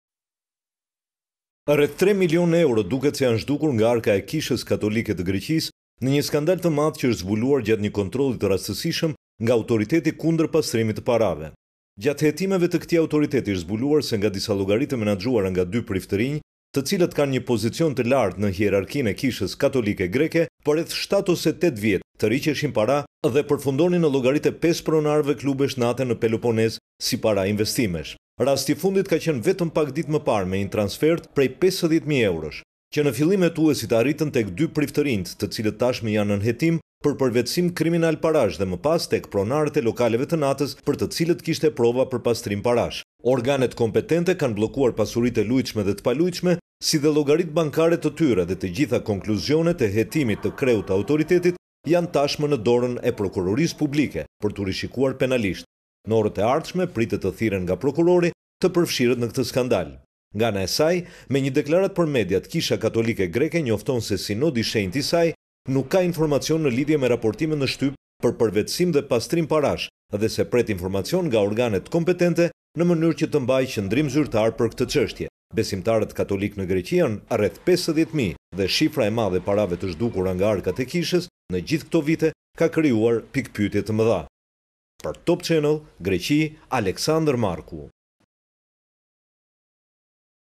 Top Channel/ Skandal! Zhduken nga arkat e Kishës Katolike 3 mln euro, priftërinjtë i investuan në...
Kisha Katolike Greke është përfshirë në një skandal, ku dy priftërinj kanë marrë 3 milionë euro nga arkat e saj.
Një hetim prokurorial për përvetësim dhe pastrim parash kundër dy figurave të larta në hierarkinë e Kishës Katolike të Greqisë dhe pesë pronarëve të klubeve të natës në zonën e gjerë të Peloponezit u hap pas një konstatimi të Autoritetit Kundër Pastrimit të Parave, sipas medias greke.
Çështja u zbulua pas një kontrolli të rastësishëm ku u konstatua se shuma të mëdha parash, që arrinin afërsisht 3 milionë euro, u zhdukën nga arkat e Kishës Katolike dhe përfunduan si… investime në lokalet e natës.
Kreu i Autoritetit, Charalambos Vourliotis, tashmë ka proceduar me ngrirjen e llogarive bankare dhe pasurive të pesë pronarëve të klubeve të natës.
Mësohet se historia nis tetë vite më parë, kur u zbuluan shuma të mëdha parash që dilnin nga llogari të caktuara të saj që operonin nga dy priftërinjtë dhe përfundonin në llogaritë e pronarëve të disa klubeve të natës dhe shfaqeshin si investime në ato biznese.
Mobilizimi i Autoritetit u ngrit pas “këmbanës” që ra në banka për lëvizjet e shumave të mëdha.
Hetimi disa mujor u krye në fshehtësi të rreptë.
Autoriteti dyshohet se konstatoi se shuma prej 3 milionë eurosh ishte përvetësuar nga fondet e Kishës Katolike.
Hetimet zbuluan destinacionin e parave, ku shuma e fundit e dyshimtë u zbulua pak ditë më parë dhe përfshinte 50 mijë euro.
Me urdhër të zotit Vourliotis, llogaritë bankare dhe pasuritë e pesë biznesmenëve të dyshuar si përfitues të shumave të mëdha janë bllokuar.
Tashmë, konkluzioni është në dorën e prokurorit që do të hetojë mundësinë e kryerjes së krimeve të përvetësimit për dy priftërinjtë dhe pastrimit të parave për pesë biznesmenët.
Të përfshirët pritet të thirren për shpjegime në mënyrë që prokurori të vendosë nëse do t’i ndjekë penalisht.
Pamjet: AFPTV/  Foto1, St.John Vianney Lay Division/  Foto2, encyclopediaofukraine/ Foto3, catholicnewsagency/ Foto4, Gilroy Dispatch/ foto5, unsplash/ foto6, Our Sunday Visitor/ foto 7, amazon ca